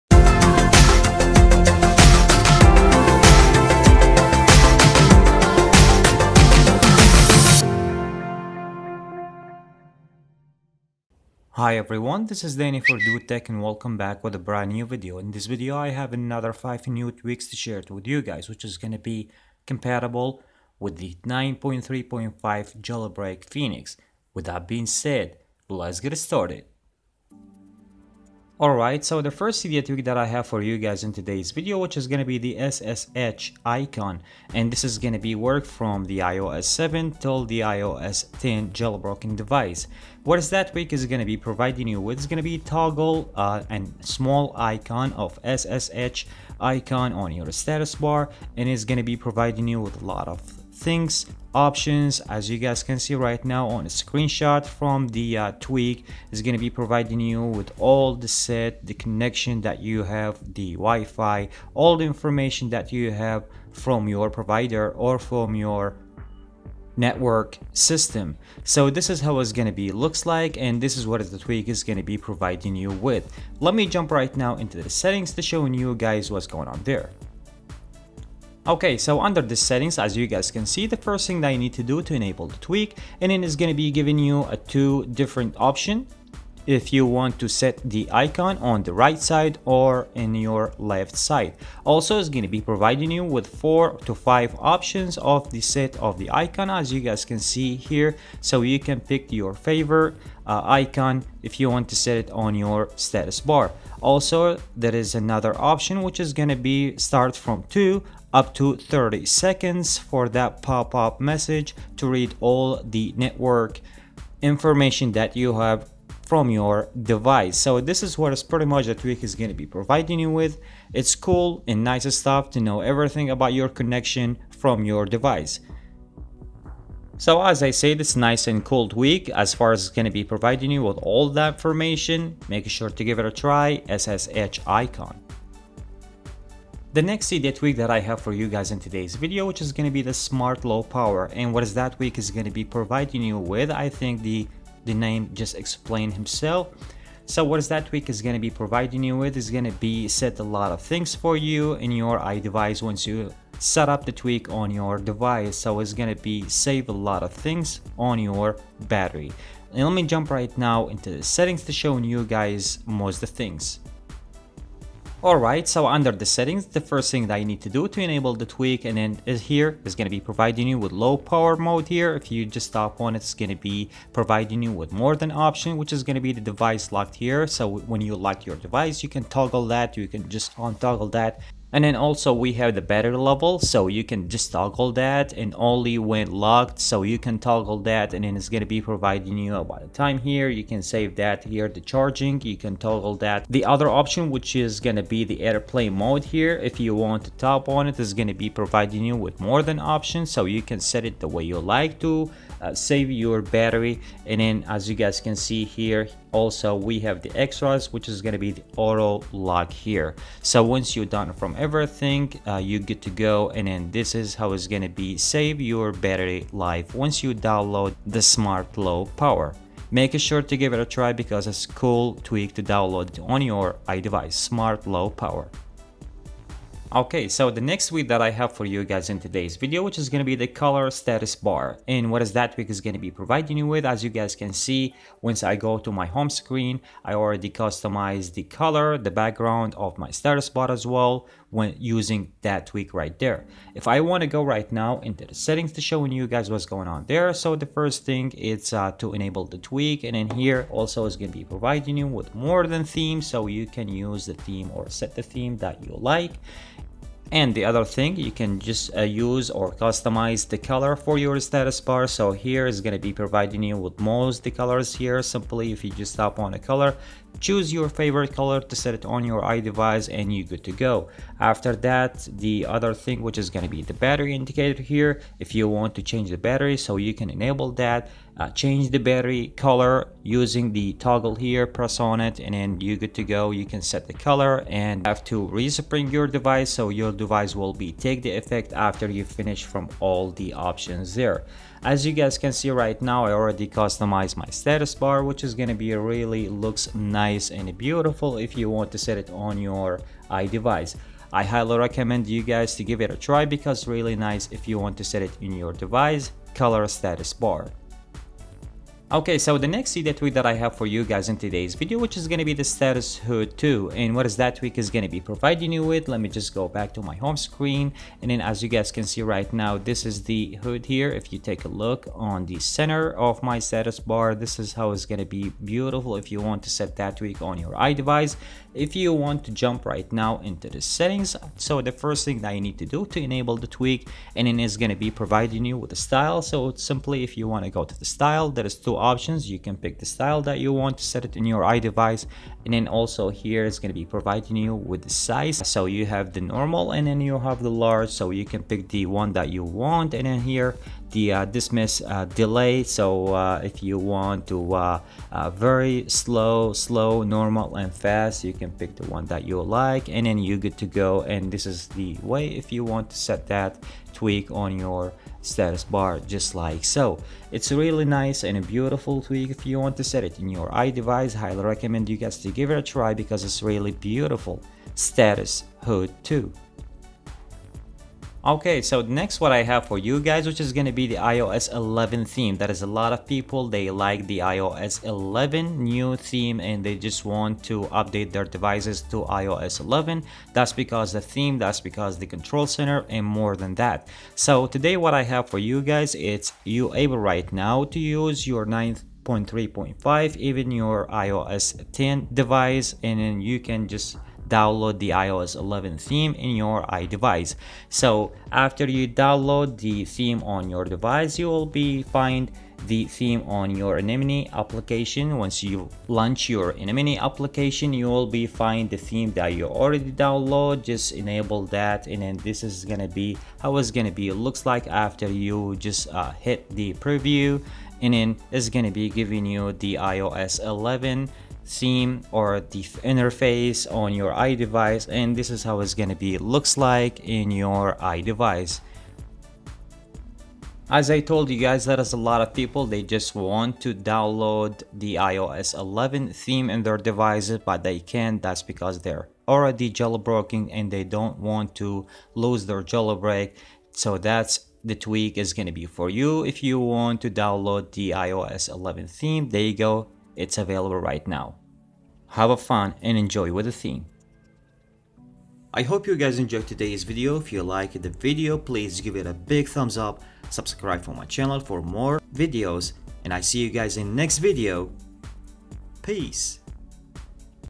Top New IOS 9.3.5 Cydia Tweaks / 2017
Hi Guys! In today's video i have more new tweaks to share with you !
Tweaks as below :

1. SSHicon
2. SmartLowPower
3. Color Status Bar
4. StatusHUD 2
5. Ios 11 Theme